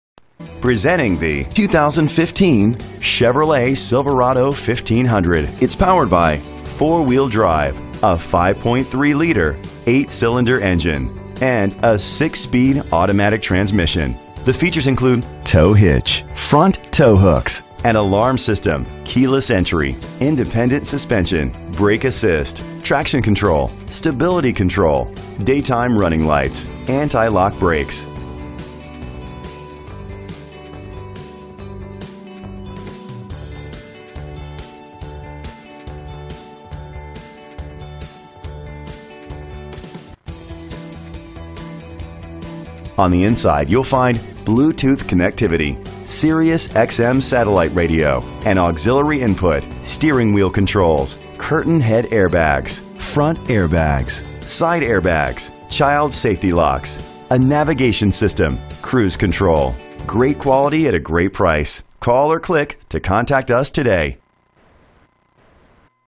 2015 Chevrolet Silverado 1500 NCT15042 - Newport NH
Year: 2015
Make: Chevrolet
Model: Silverado 1500
Trim: Work Truck
Engine: 5.3 liter 8 cylinder 4WD
Transmission: 6-Speed Automatic
Color: Black
Mileage: 1
Address:
VIN:1GCVKPEC8FZ165228